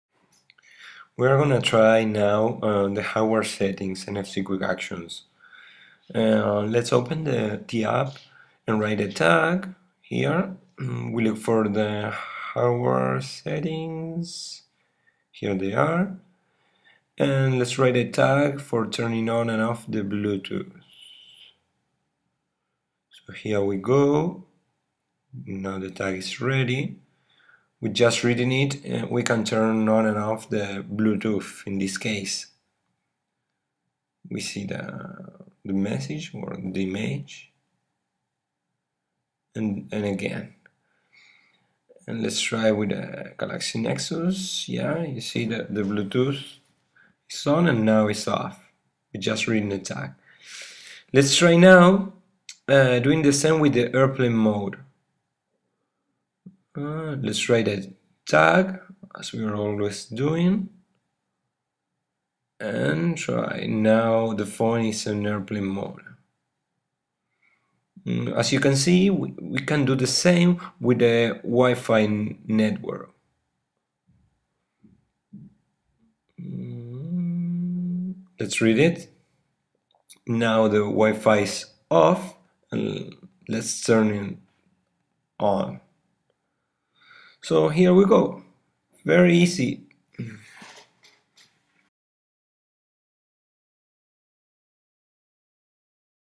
Turn on/off Bluetooth, Wi-Fi and Airplane Mode using NFC
In this video we can see to turn on and off different hardware available in our smartphone. We are using NFC Quick Action Application for Android. Really easy: just write the tag with the action you want to execute when reading it. Enjoy!!!